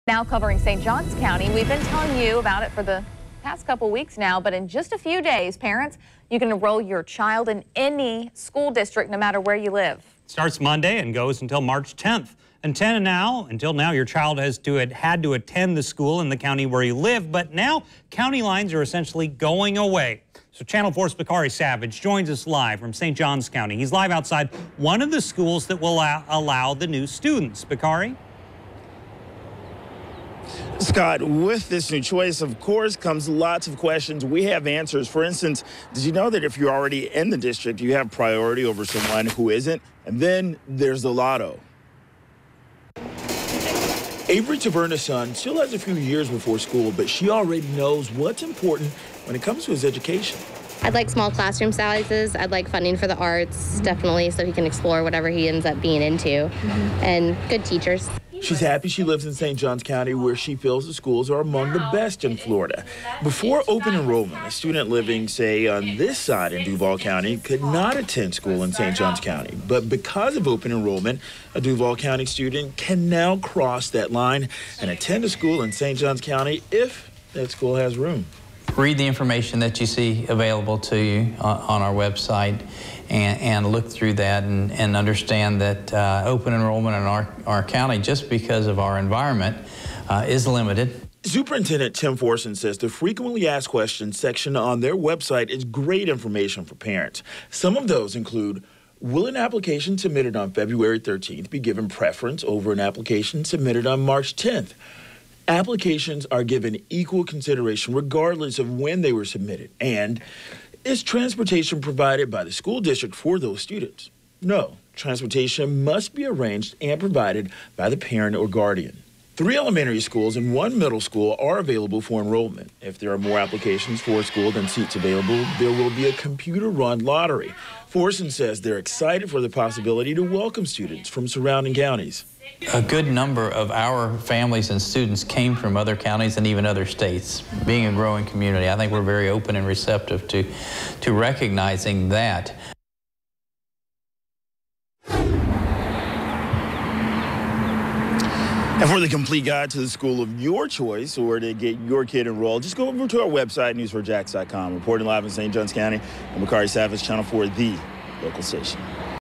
SJC school choice open enrollment
VIDEO: Beginning on Monday, open enrollment starts for St. Johns County.